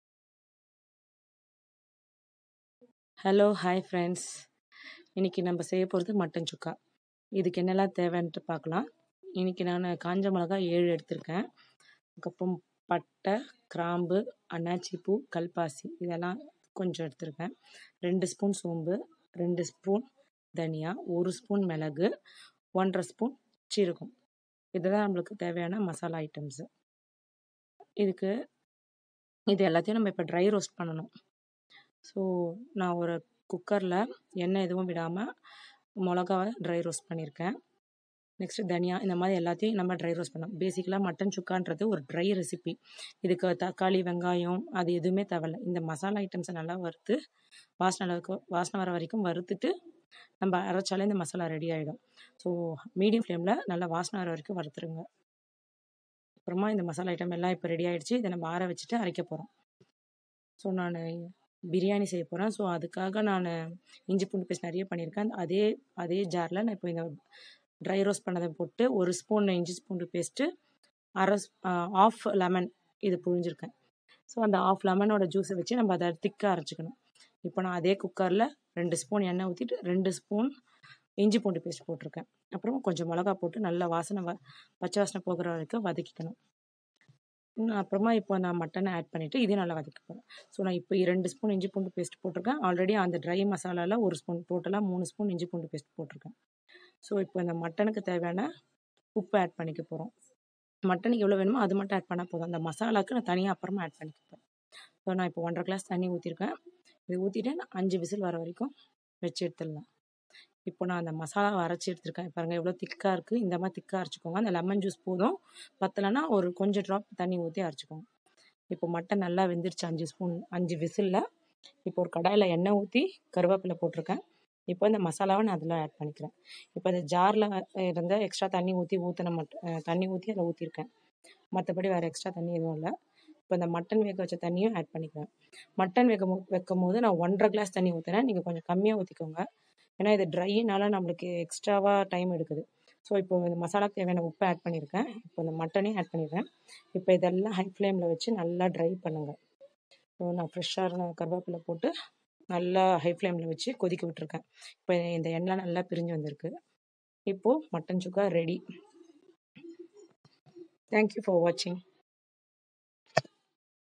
Mutton chukka/No onion No tomato/spicy mutton chukka/dry roast mutton fry/chettinad spicy mutton/
Hi friends. Today I have posted MUTTON CHUKKA RECIPE... its dry roast spicy dish. very yummy dish ... try it once and for comment

Ingredients
Mutton 1/2kg
Lemon 1/2
Salt as per taste
Curry leaves

To grind

Cloves,star,cinnamon few as per ur taste
Pepper - 1tsp